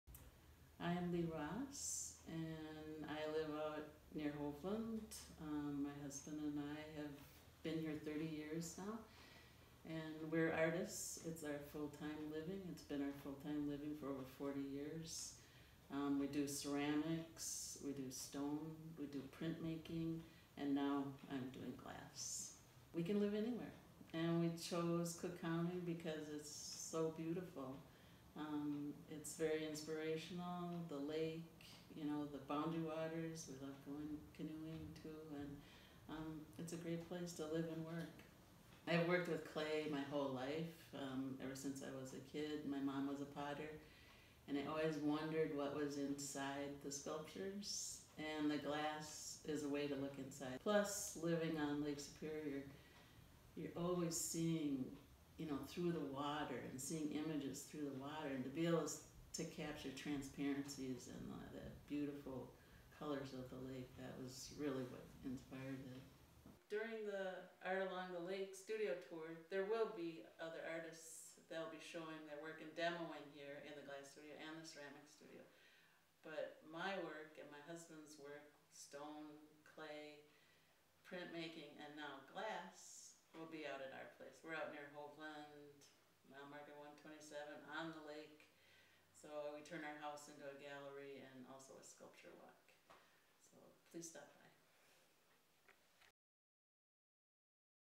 Interview with artist Lee Ross about the upcoming Art Along The Lake Fall Studio Tour
Interview with artist  Lee Ross about the upcoming Art Along The Lake Fall Studio Tour. Lee and Dan Ross have been collaborating together for over 40 years. Their artistic mediums include ceramic, stone, printmaking and now, glass. Their Hovland home and studio is only open to the public for 10 days a year. Do not miss an opportunity to see where they work and find inspiration.

Video  Lee Ross about Art Along The Lake   HD